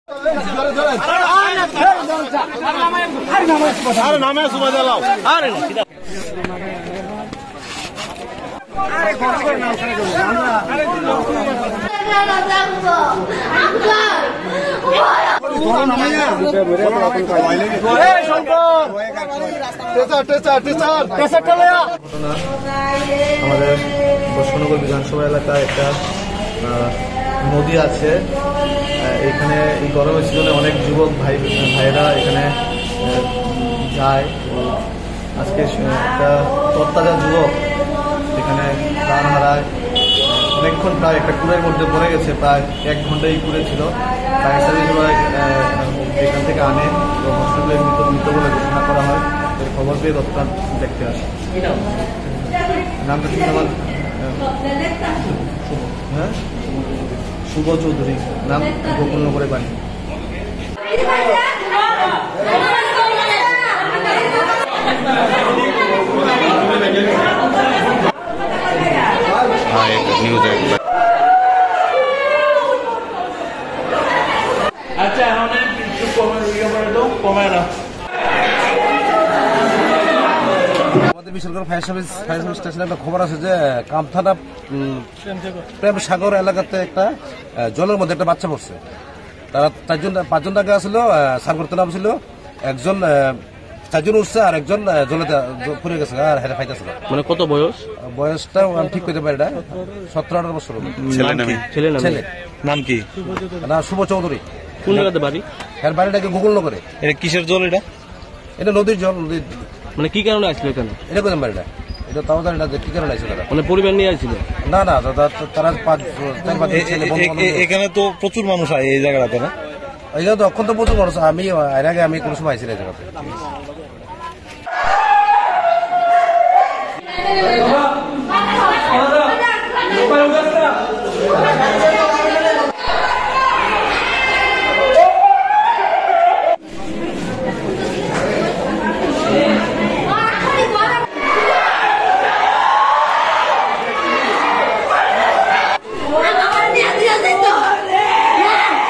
বন্ধুদের সাথে স্নান করতে গিয়ে জলে ডুবে মৃ*ত্যু সদ্য উচ্চ মাধ্যমিক পাশ করা ছাত্রের
বন্ধুদের সাথে স্নান করতে গিয়ে
 জলে ডুবে মৃ*ত্যু সদ্য উচ্চ মাধ্যমিক পাশ করা ছাত্রের
 ঘটনা বিশালগড় কামথানার প্রেম সাগরে মঙ্গলবার দুপুরে
 মৃ*ত যুবকের নাম শুভ চৌধুরী
 বাড়ি গোকুলনগরে